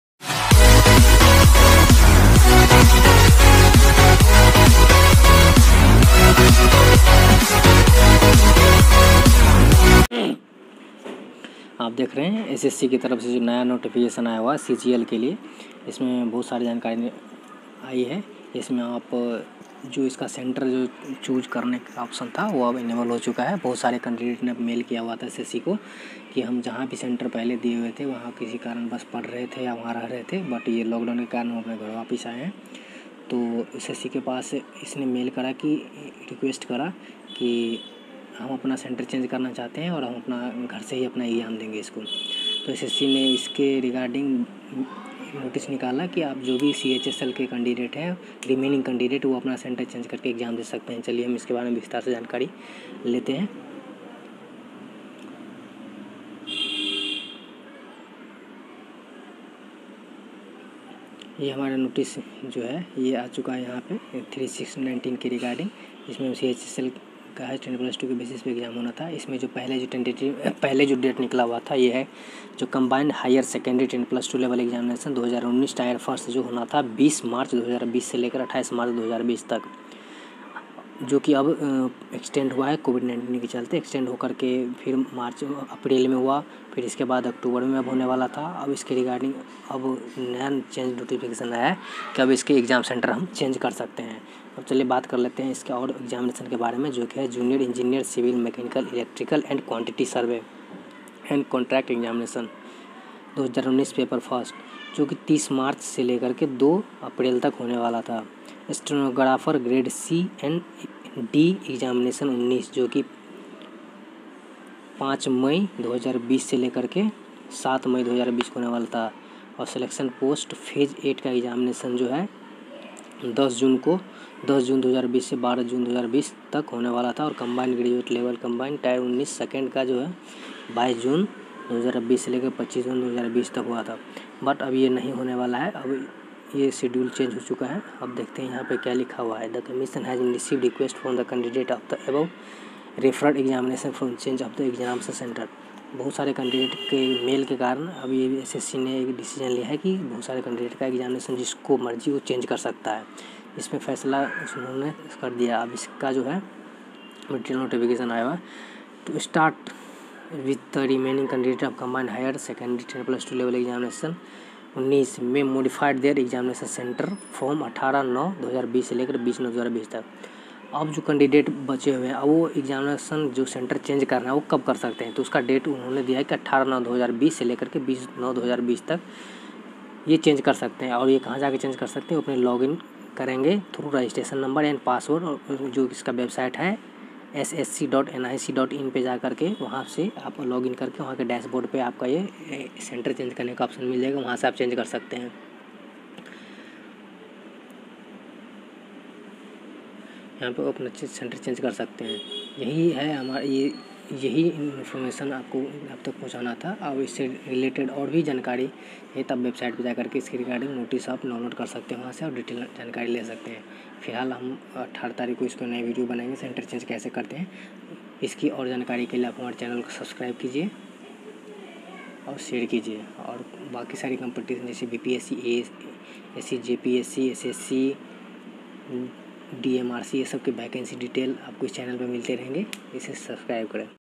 SSC CENTER CHANGE | SSC JE OFFICIAL NOTICE | SSC JE EXAM 2019-20 | BIG NEWS SSC JE | SSC JE APDATE
IS VIDEO ME BAT KATENGE KI SSC CHSL ME CENTER CHANGE KAB KARNA HAI DATE KE BARE ME BAT KARENGE
AAG EKE APDATE KE BARE ME BHI BAT KARENGE
KI SSC JE  KA CENTER BHI CHANGE HOGA KI NAHI

NEW NOTIFICATION FOR SSC ASPERANT
SSC JE EXAME CENTER CHANGE
SSC CHSL EXAME CENTER CHANGE
NEW NOTICE SOON THROUGH SSC